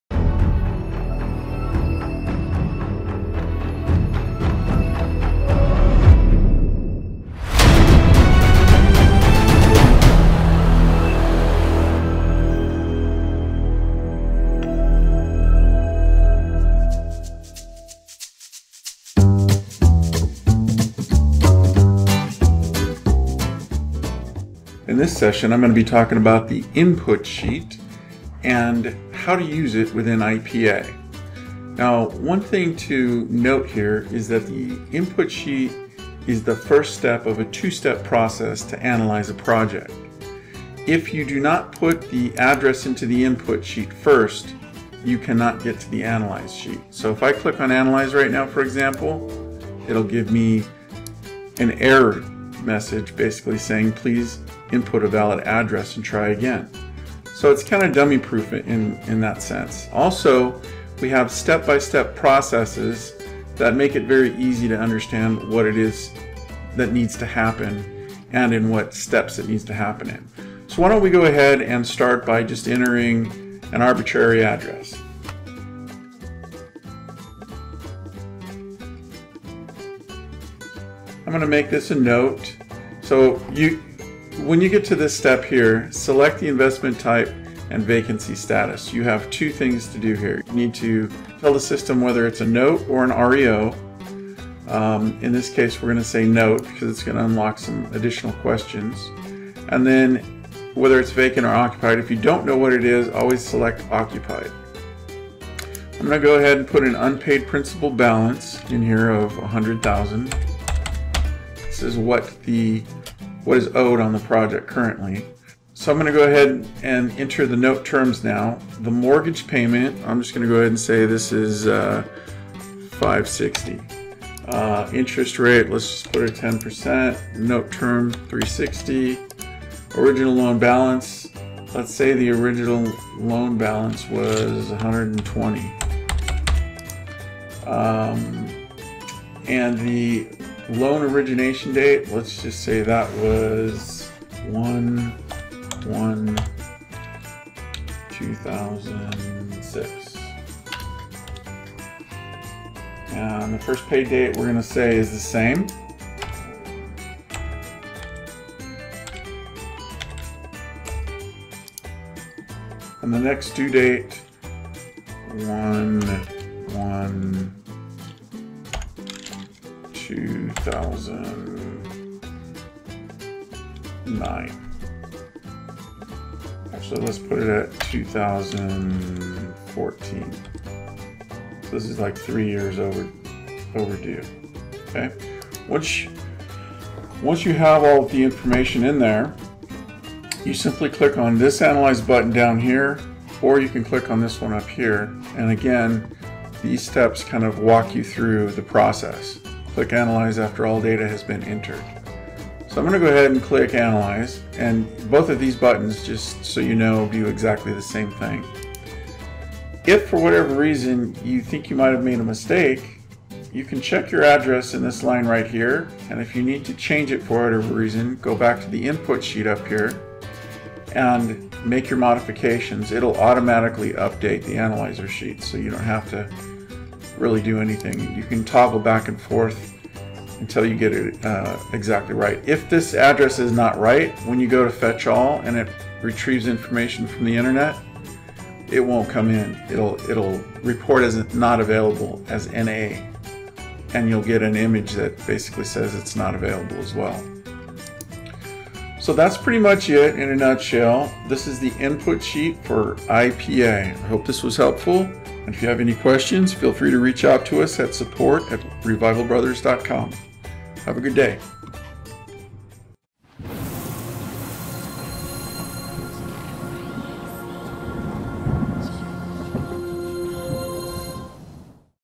IPA - Navigating the Input Page
IPA, the IDEAL Project Analyzer was created for Note and REO investors.  This is not just another excel spreadsheet. This is an excel application, with predictive analytics built in. It includes a flexible dynamic calculator, an exit strategy analyzer and an online due diligence tool that will automatically search for property related information and insert it into the spreadsheet. This is the only tool like it on the market and is well worth investigating. for more information.